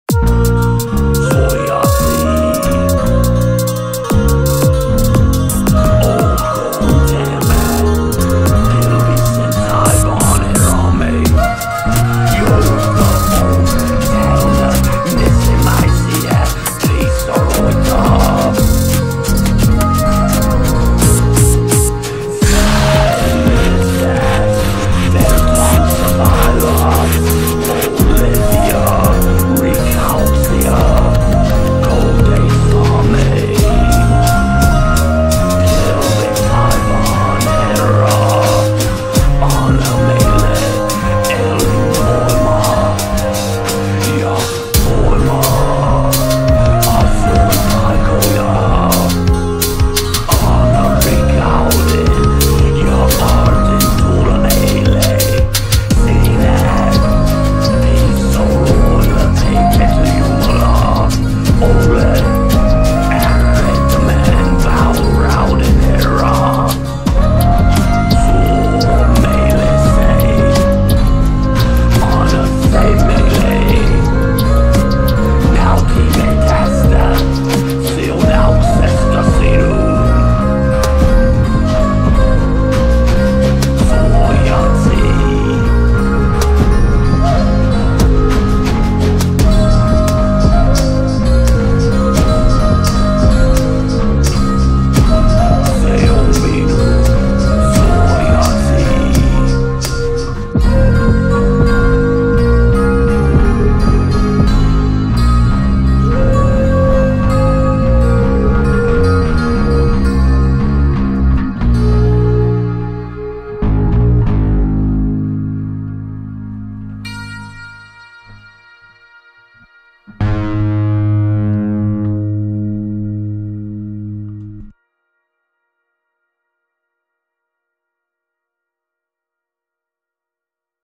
suojatti
Olkoon tämä pilvisen taivaan herramme, joka on täynnä nestemäisiä pisaroita ... säilyttää vertaansa vailla olevia rikkauksia kodeissamme. Pilven taivaan herra, anna meille elinvoimaa ja voimaa ... asuinpaikkoja. Anna rikkauden ja aarteen tulla meille. Sinä, pisaroilla peitetty Jumala, olet äärettömän vaurauden Herra. Suo meille se, anna se meille: nautimme tästä siunauksesta sinun.